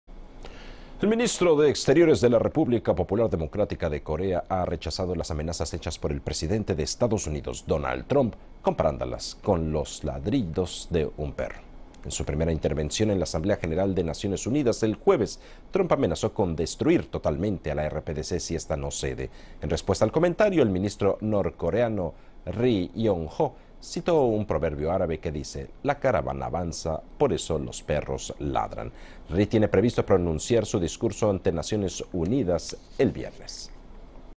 Ministro de Exteriores de la RPDC compara a la intervención de Trump con los ladridos de perro
El ministro de Exteriores de la República Popular Democrática de Corea ha rechazado las amenazas hechas por el presidente de Estados Unidos, Donald Trump, comparandolas con los ladridos de perro.